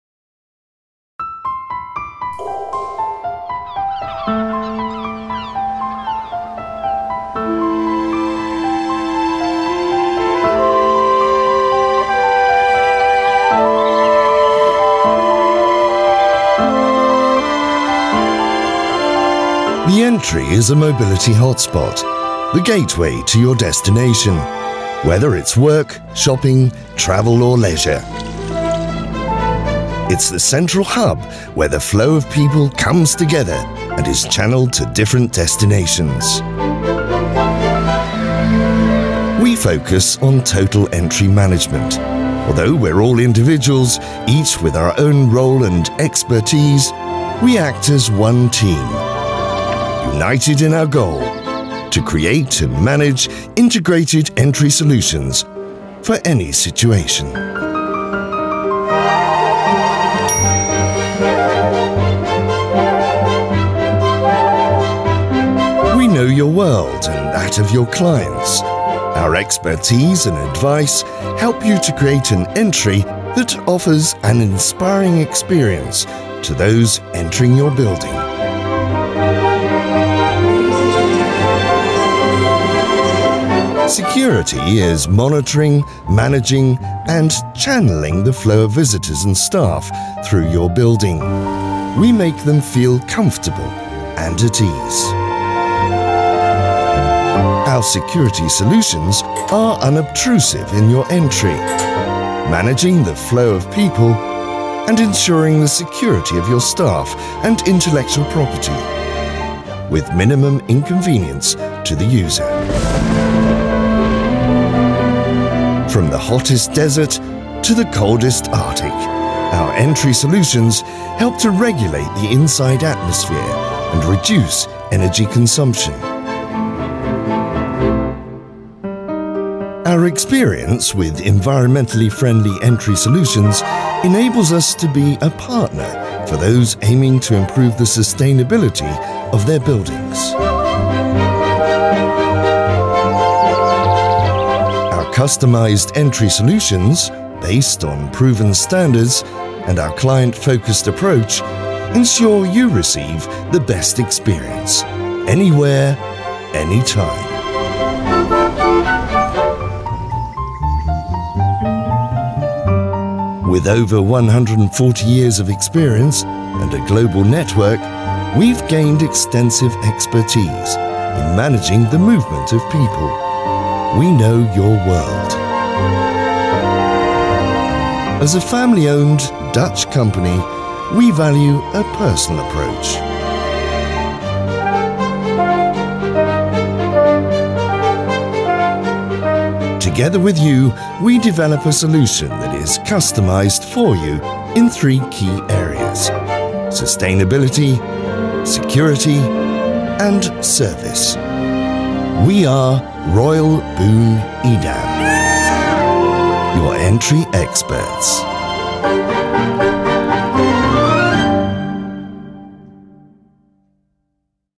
Your Entry Experts - Corporate Video | Boon Edam
Boon Edam Your Entry Experts with over 140 years of Experience

With over 140 years of experience of engineering quality and a global distribution network, Royal Boon Edam can offer you expertise and knowledge as well as future proof entry solutions that meet your specifications and requirements. As the world increasingly revolves around access, and entries become mobility hotspots, our world revolves around yours. We fit the puzzle in the development of your building, creating entries at the intersection of sustainability, security and service

Mobility Hotspot
With work environments becoming increasingly dynamic, the entry to a building has become the gateway to a visitor's destination: the hospital's entry is the first step towards recovery and health; the entry to the building where you work a gateway to personal fulfilment; the airport terminal the threshold to relaxation and rest on vacation; the shopping centre's atrium a portal to enjoyment and a new wardrobe.

The entry is where visitors leave behind the public outside world and are immersed in your world. It's the mobility hotspot where visitors with different destinations cross paths and find their way to their future.

We are the Entry Experts channeling the flow of people in and around buildings creating entries at the intersection of sustainability, security and service